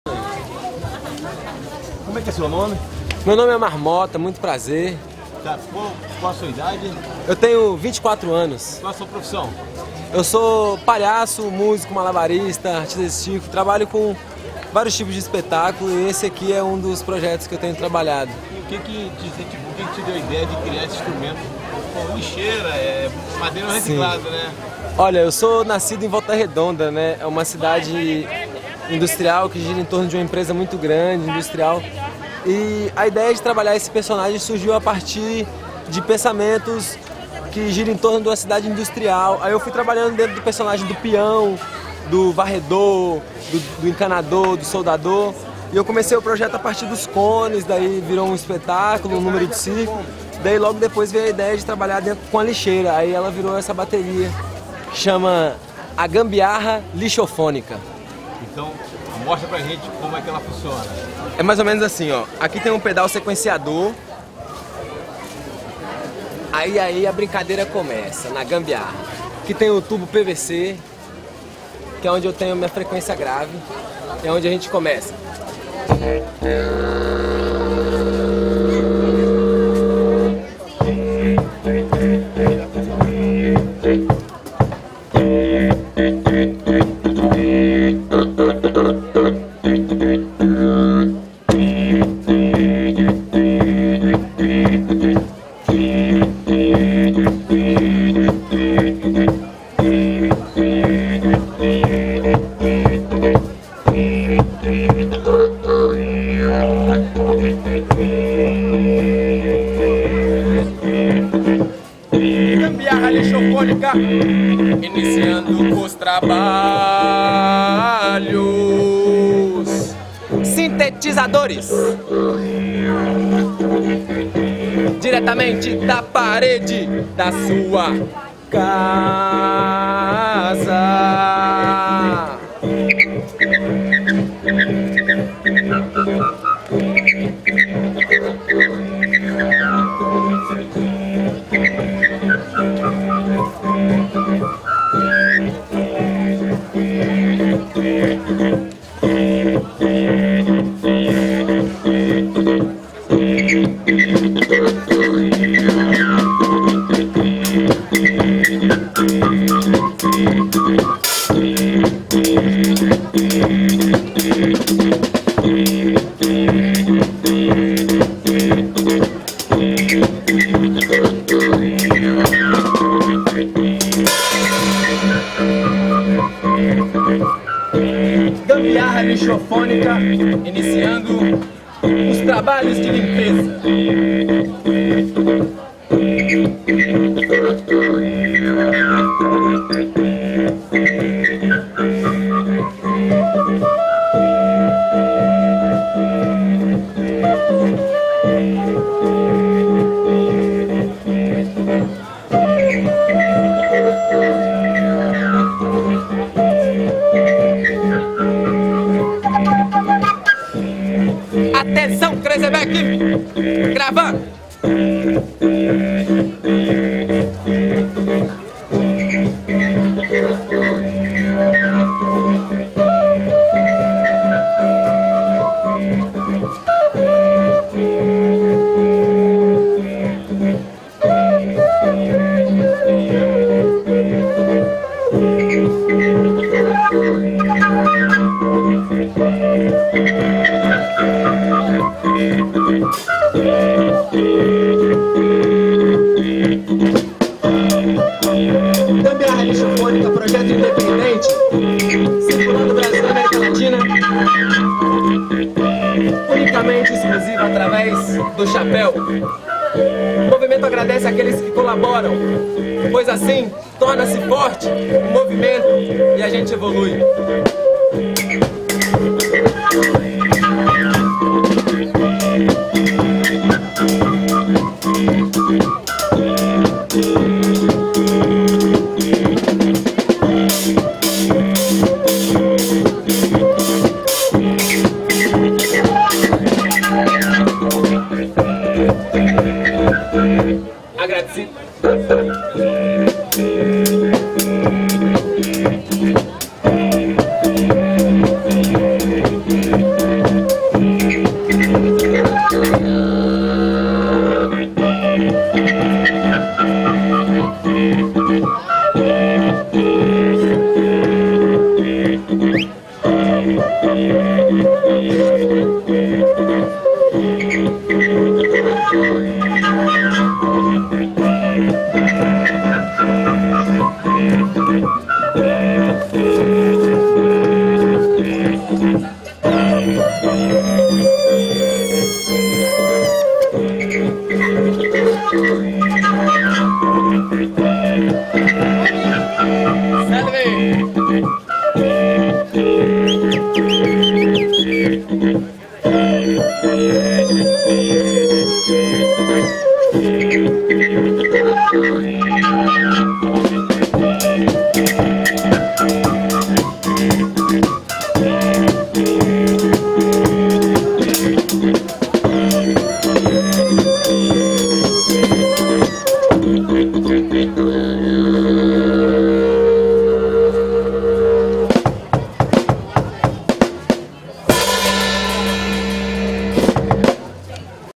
Artista de rua em Paraty
--Um artista de Rua morador de Paraty Cidade Histórica do Rio e Janeiro dando um Show nas ruas de Paraty com sua invenção  com uma lixeira e tubos de PVC.